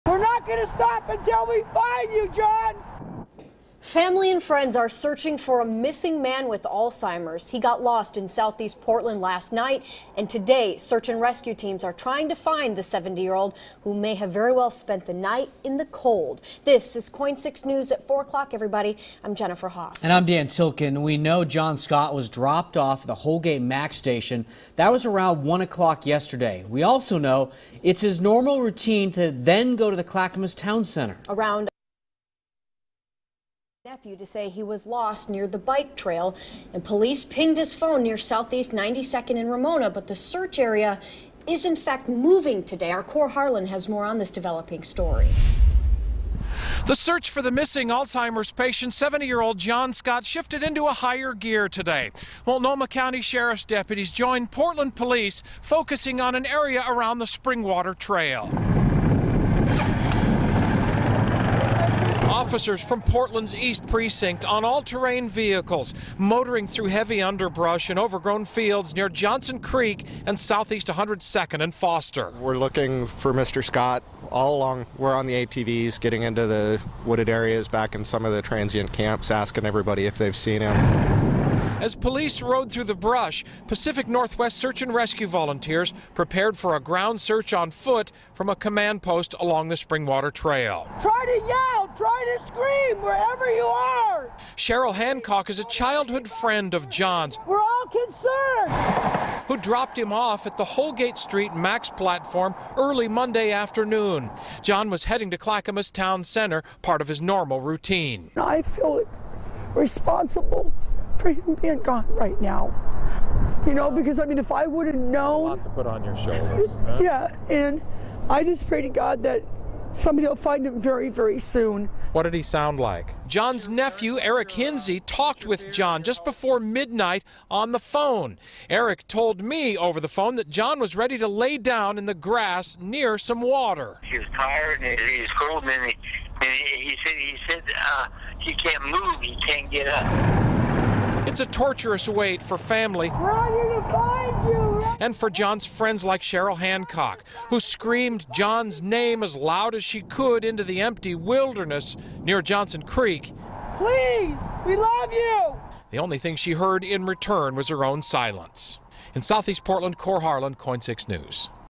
Alzheimer's patient, 70, missing in SE Portland
John Scott, 70, has not been seen since he was dropped off at a MAX stop at 1 pm Monday.